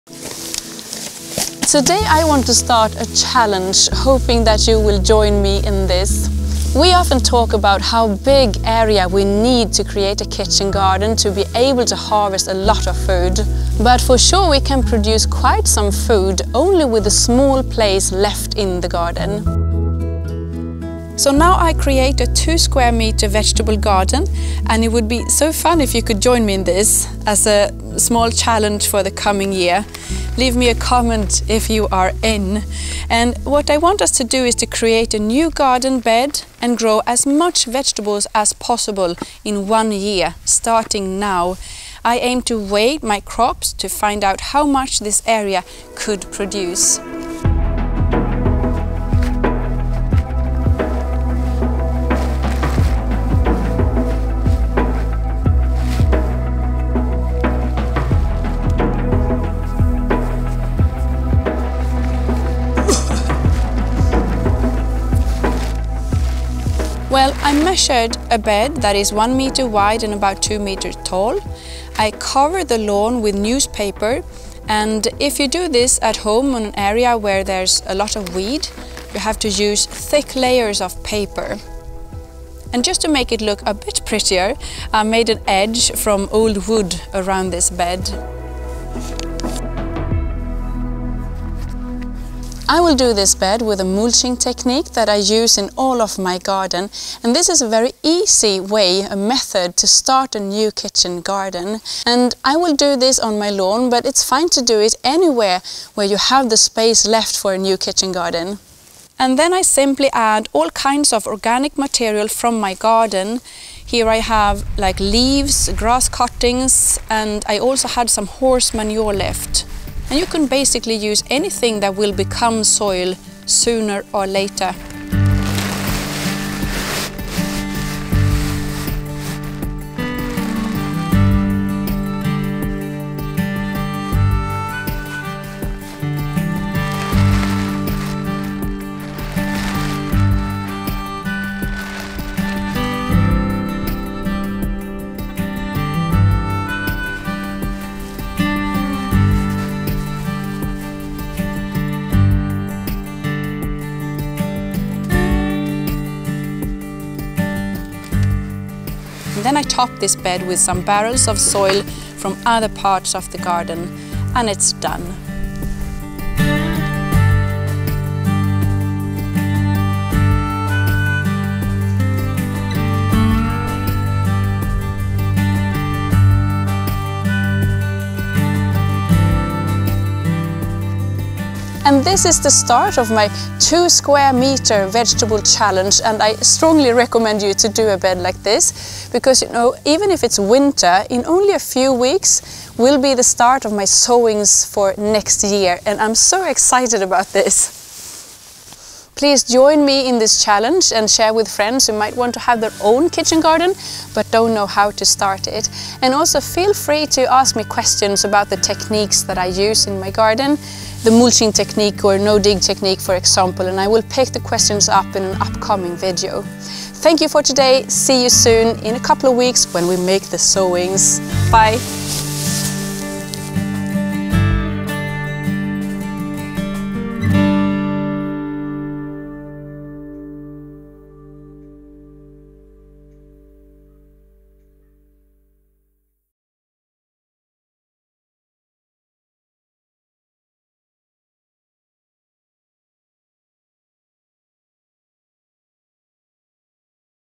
20 Square Feet Vegetable Garden Challenge – in a raised bed: Mulching techniques (part 1)
We often talk about how big kitchen garden we need to be able to get a big harvest, but for sure you can produce a lot of food in only a small space! In this challenge, I will grow food in a all new 20 square feet bed to see how much it can produce. It would be so fun if you would like to join me in this challenge, so we can explore the opportunities together! Please comment if you’re in.